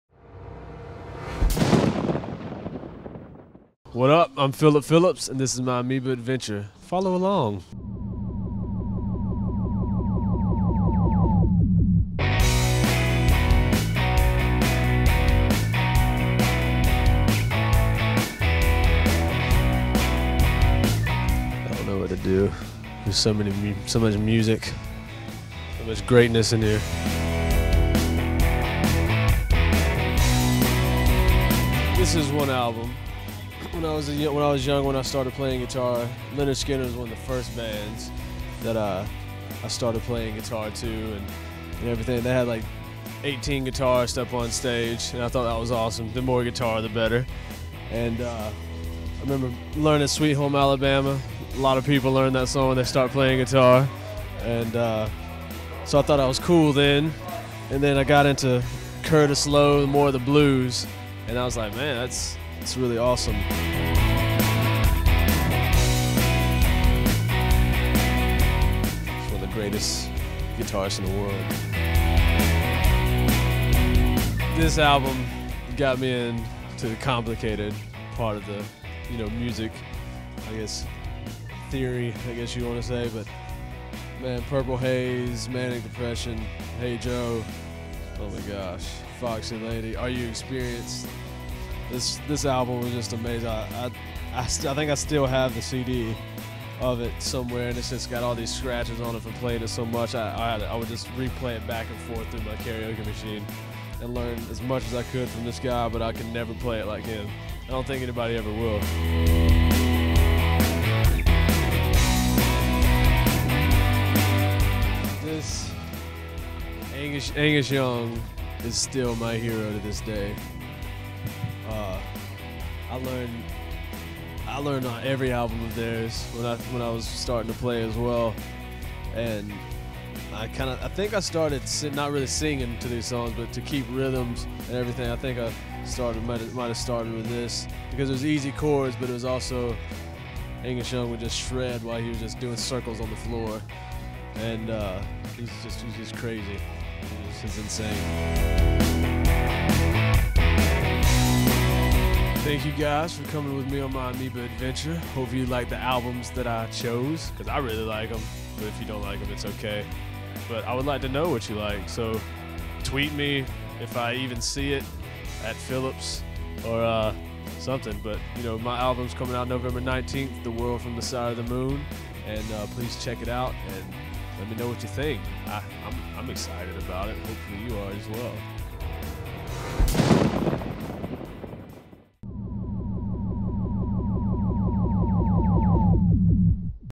Buzznet Amoeba Adventures:Phillip Phillips
Phillip Phillips takes Buzznet on an Amoeba Adventure!